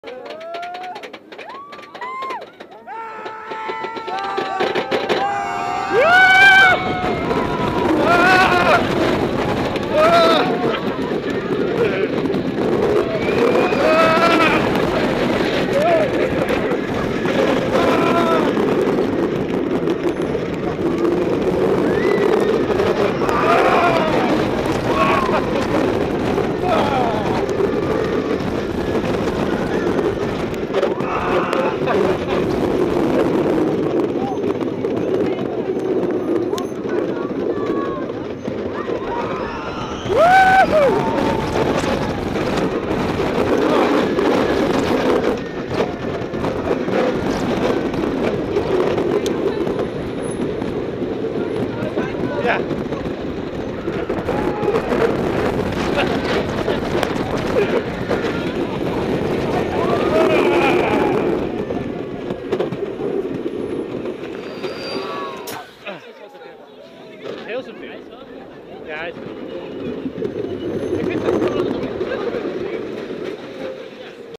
Walibi
Opgenomen in Walibi.
Gefilmd met de camera op mijn rugzak bevestigt.
Camera: ContourHD1080p